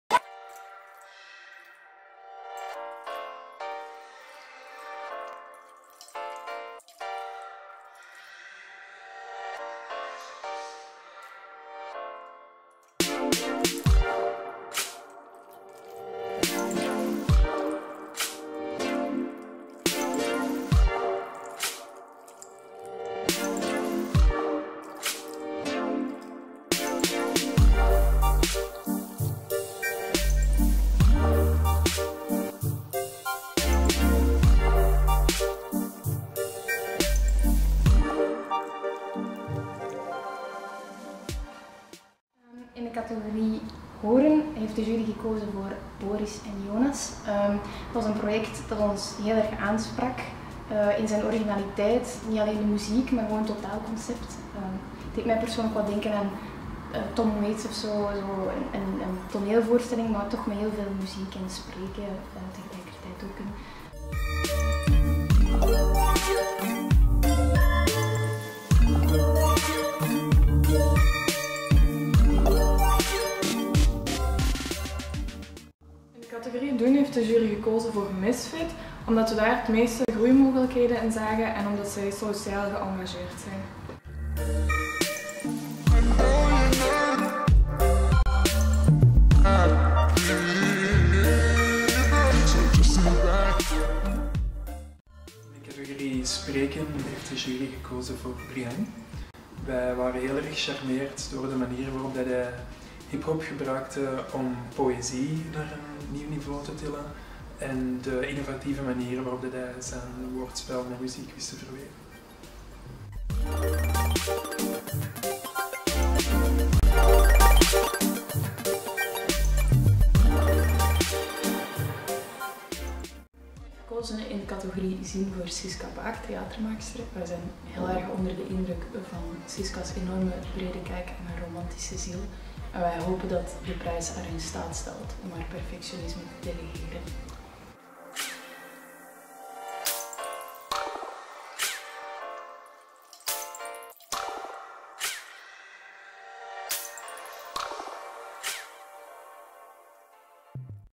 BILL Awards: het verdict van de jury.
Hiphoppers, cartoonisten, slam poets, character artists, videokunstenaars ... Het wemelt van de creatieve jongelui in Vlaanderen en Brussel. Tijd om hen extra in de spotlight te zetten met de BILL Awards.